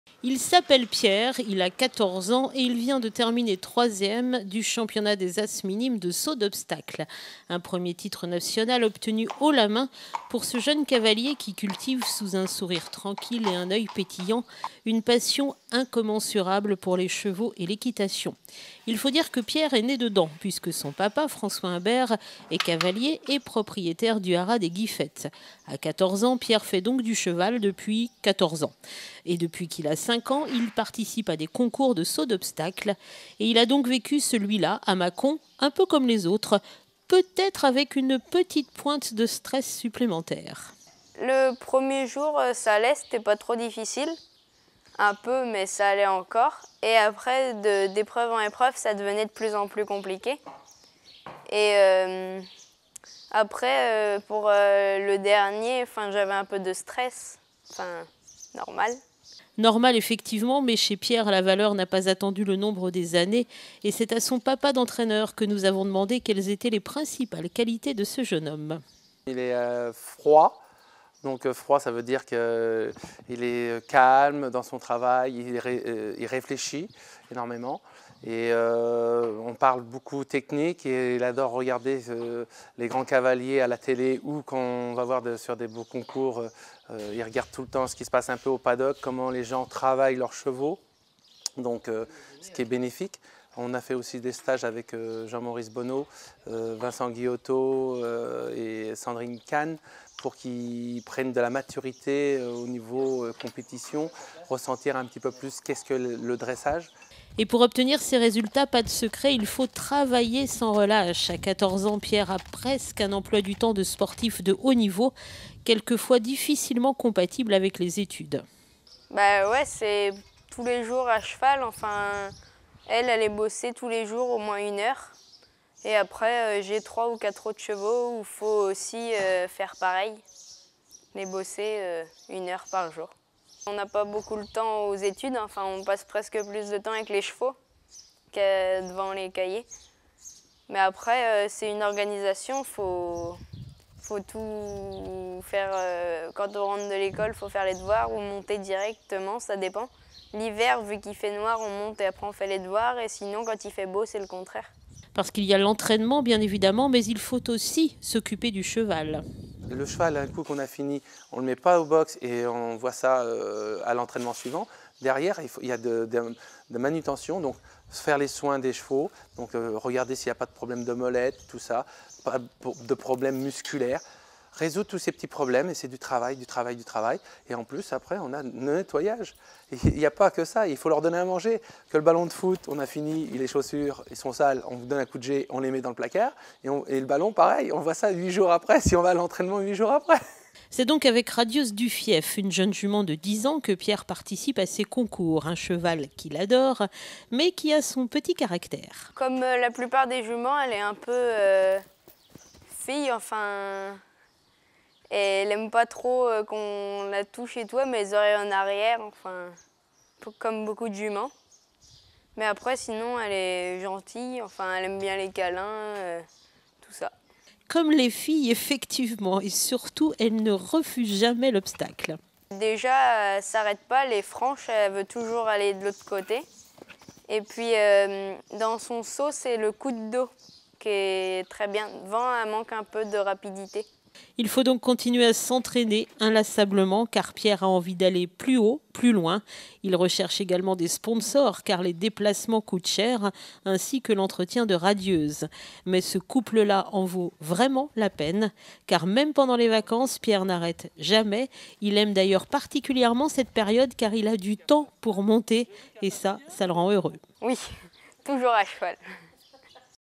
Luçon : Pierre Imbert, un as de l'équitation !
Il a tout juste 14 ans et vient de prendre la 3ème place du championnat de France de saut d'obstacles. Pierre Imbert est de la graine de champion et il espère aller loin dans cette discipline difficile qu'il aime par dessus tout. Portrait.